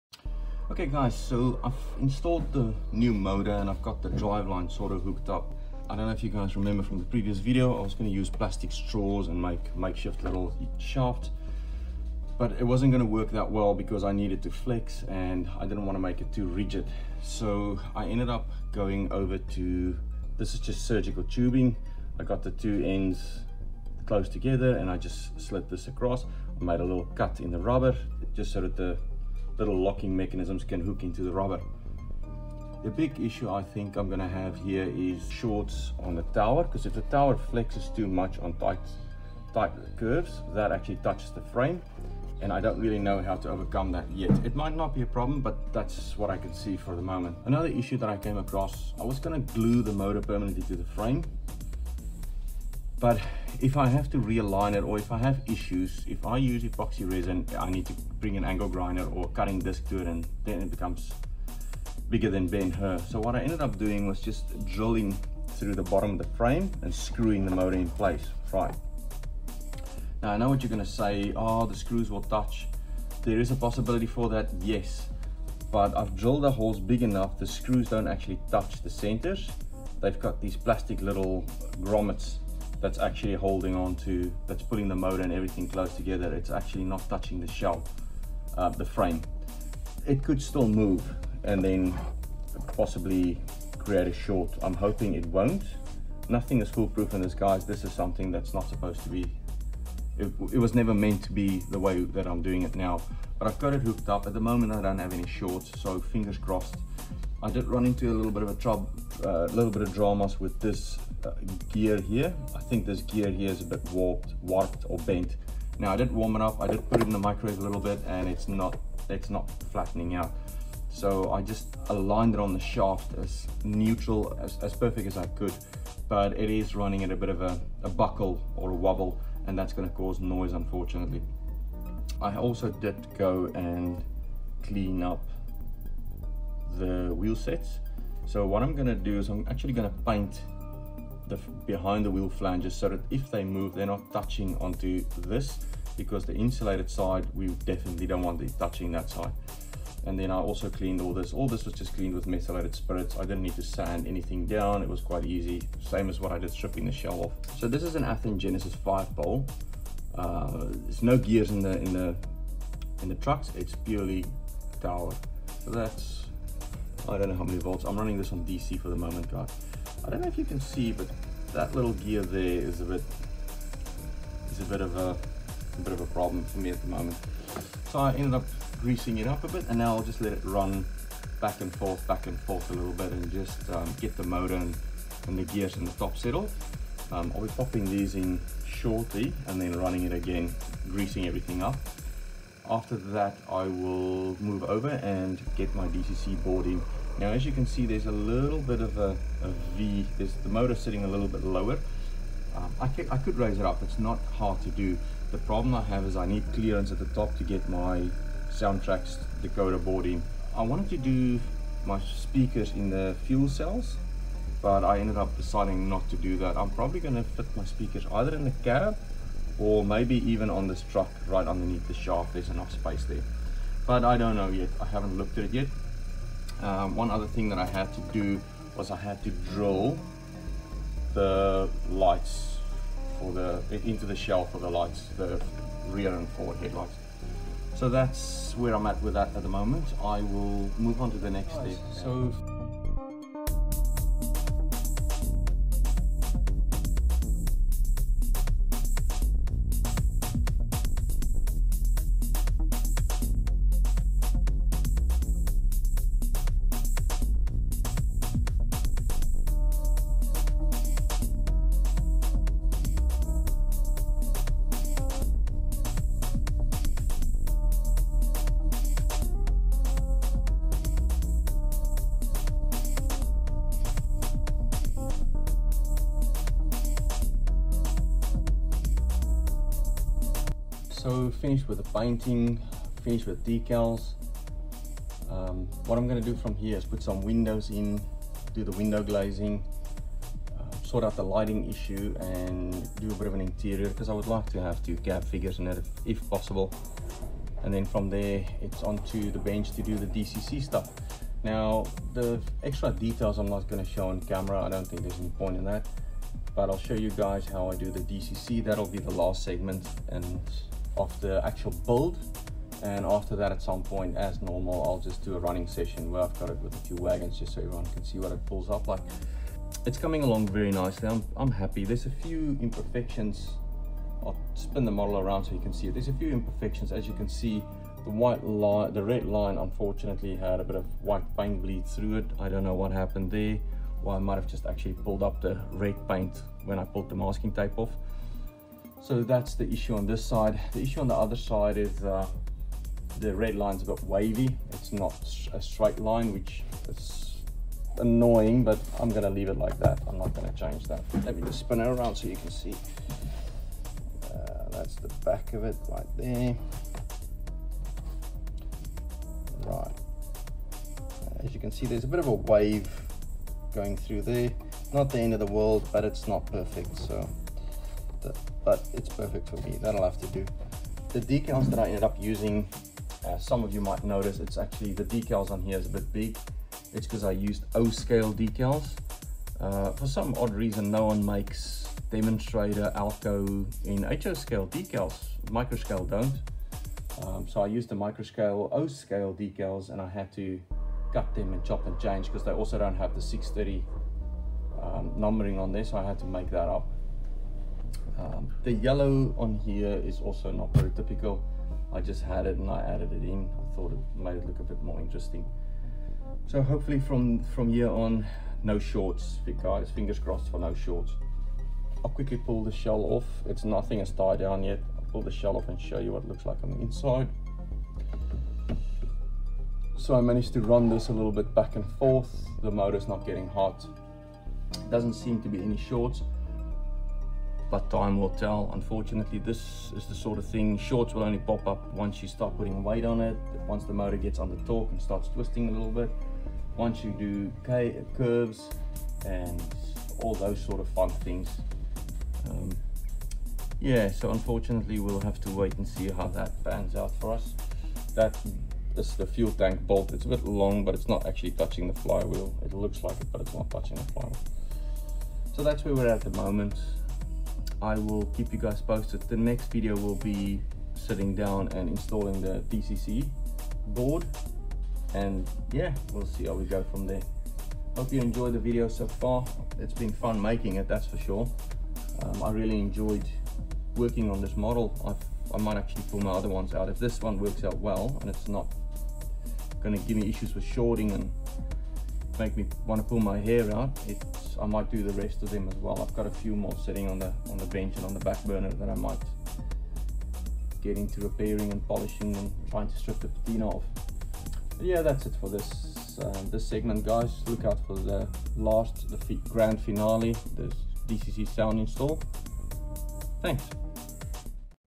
Rebuilding Ho brass locomotive
In part 4 i re-power, assemble, paint and apply decals to the Alco C630 project. Few slide shows to show the painting side of things.

Hopefully i sealed all the contact points well enough to avoid future shorts.
 Few flaws on the painting side but for the most part i have learned heaps.
This project is coming along nicely.
Hope you enjoying the journey with me.